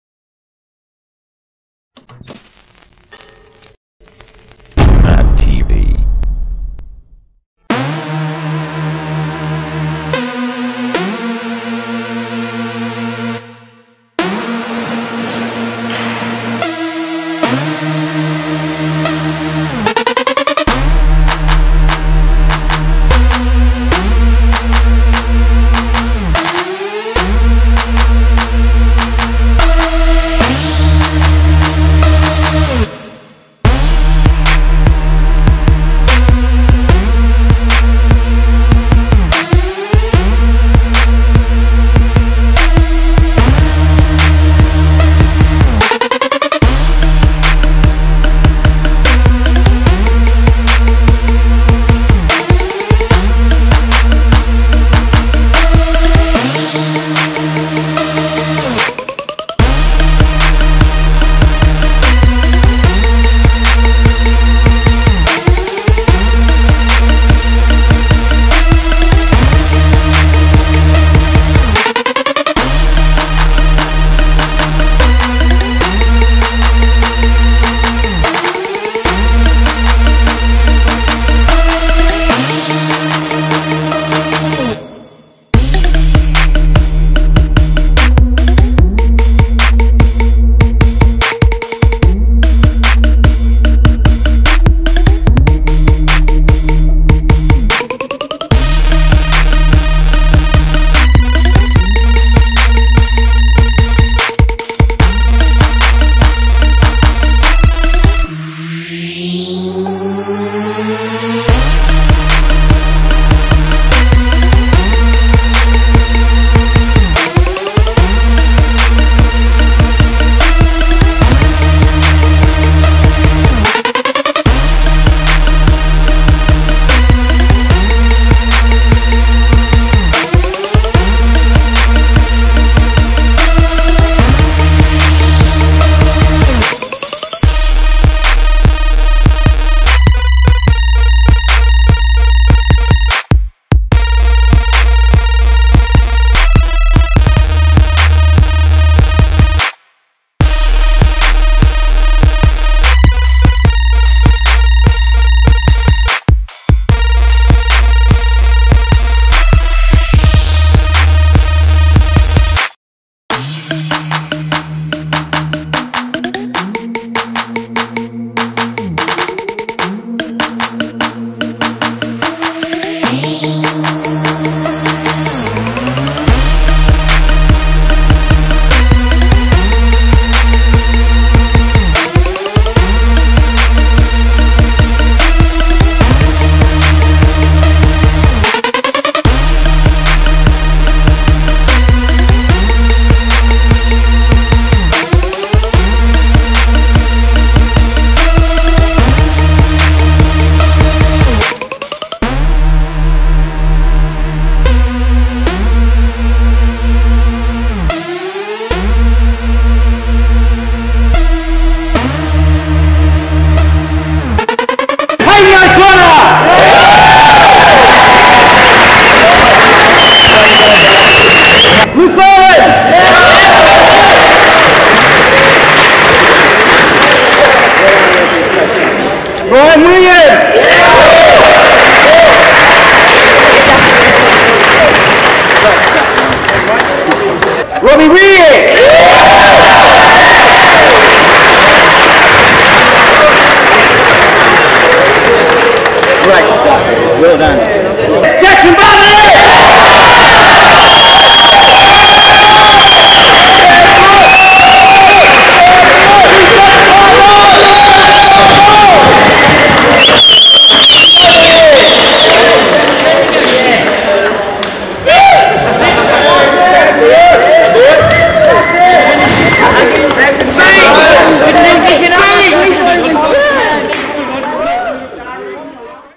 ISA Australian Titles 2012 | GC Compound
The ISA Australian Titles were held on Monday the 10th of June 2012 at GC Compound, QLD, Australia. Billy Watts & Matty Ceravolo are both currently injured and could not compete but the rest of the team qualified for the international titles in the UK later this year (October.) Jackson Bartlett & Robbie Weir made the podium taking 1st and 3rd place!